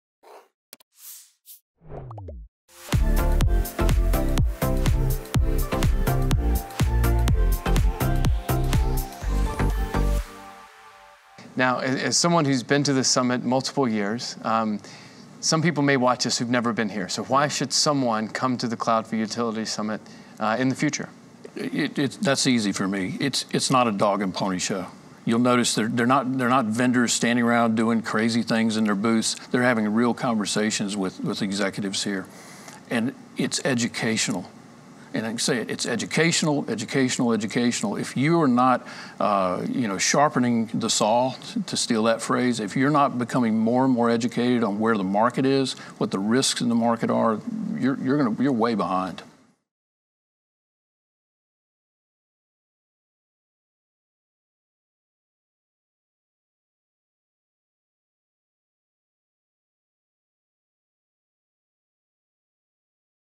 In Conversation: Edwin Crow, AAC Utility Partners & Amos Snead (Teaser C)
Edwin Crow On Why You Should Attend Cloud Summit